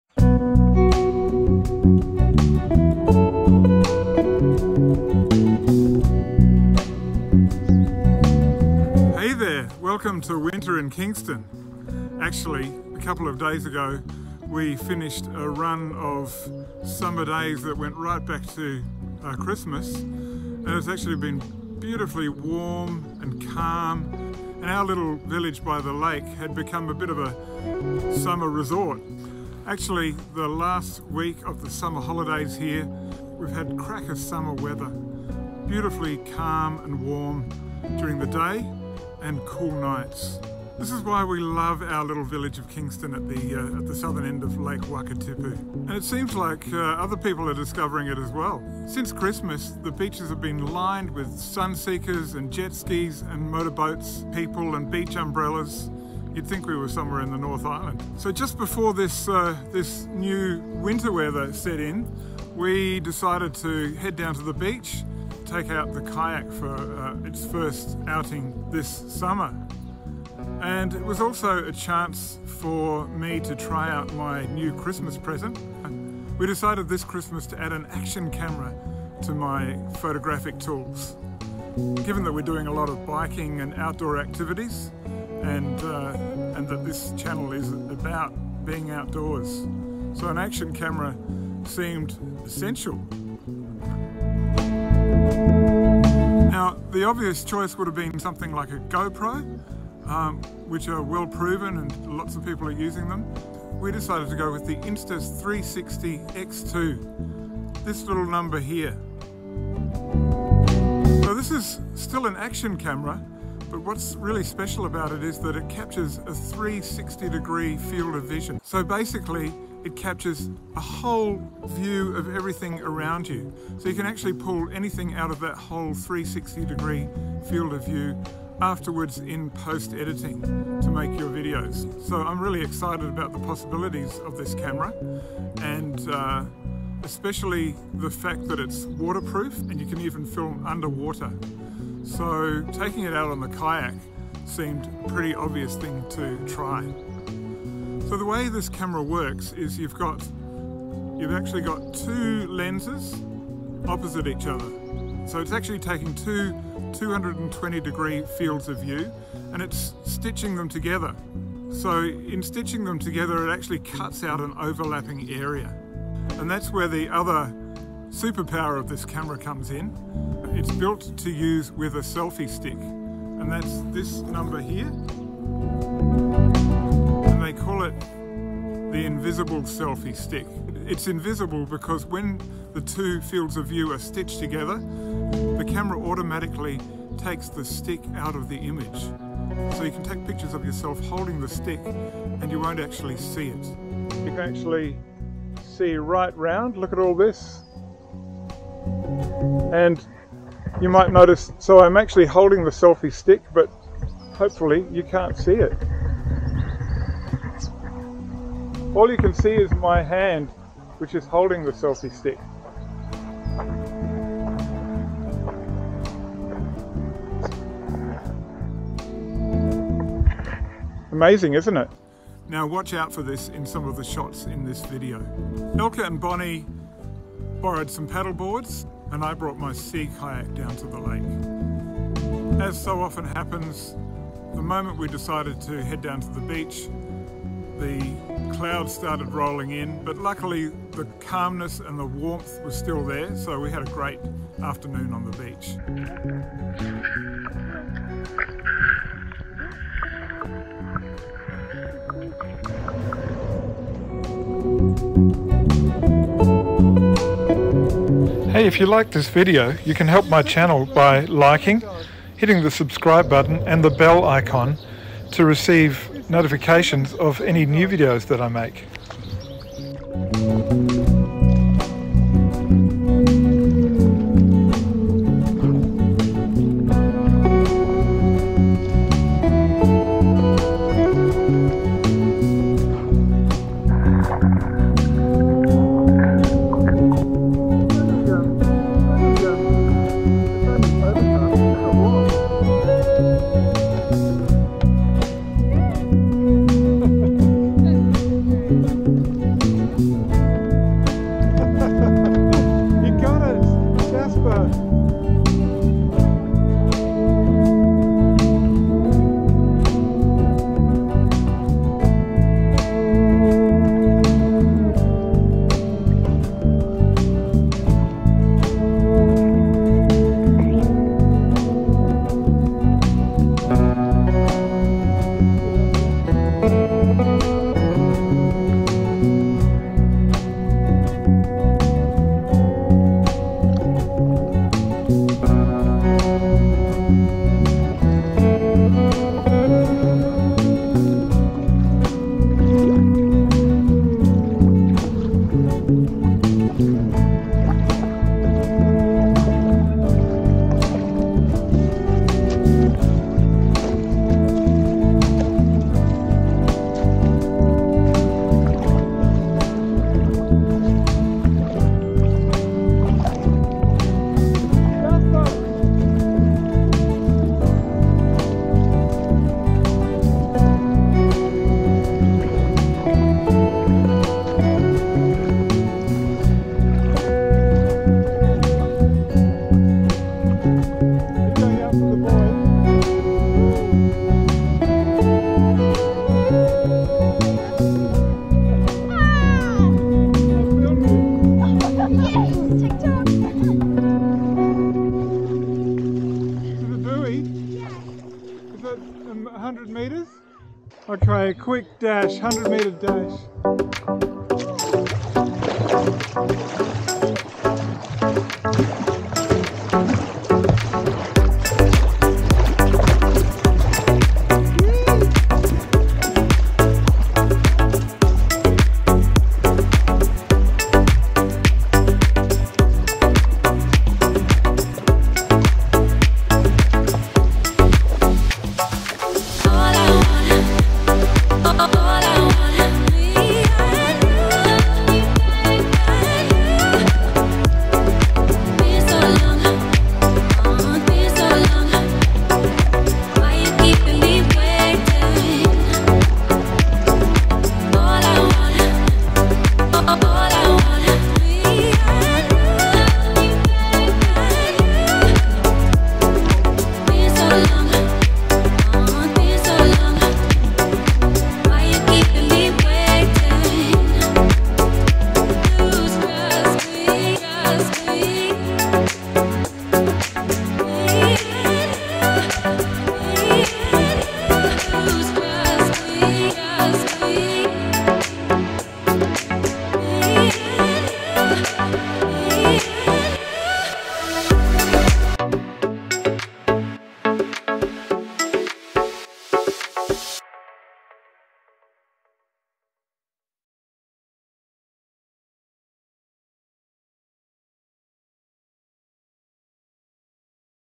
A Day at the Beach with my Kayak and Insta360 One X2 Camera
Our little village at the Southern end of Lake Wakatipu has become a go-to place for beach fun and boating in the south of the island. After a run of perfect beach days to start the year the clouds finally rolled in. It was still warm and calm so I pulled out the sea kayak for the first time this year. This was an opportunity to experiment with the creative possibilities of my new Insta360 One X2 Camera.